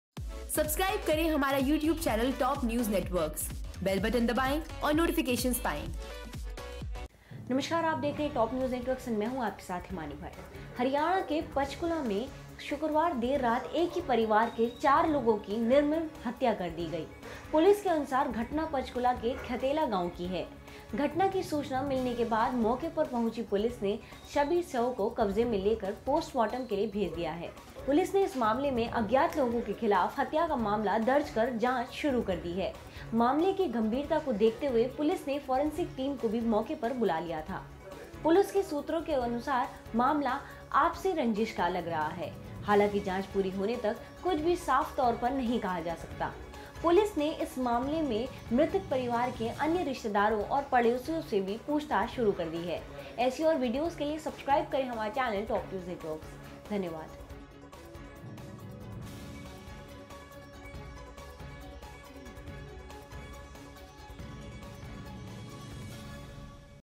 4 Members Of Haryana Family Found Dead Under Mysterious Circumstances | Top News Networks
Four members of a family, including two women, were found dead under mysterious circumstances at their house in a village in Panchkula district of Haryana.